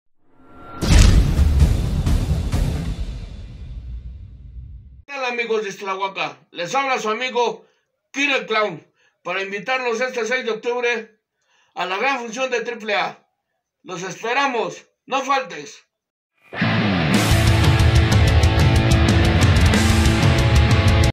INVITACION KILLER CLOWN
KILLER CLOWN INVITA ESTE 6 DE OCTUBRE AL SALON IXTLAHUACAN DE IXTLAHUACA A LA FUCION TOTALMENTE GRATUITA CON ESTRELLAS DE LA CARAVANA ESTELAR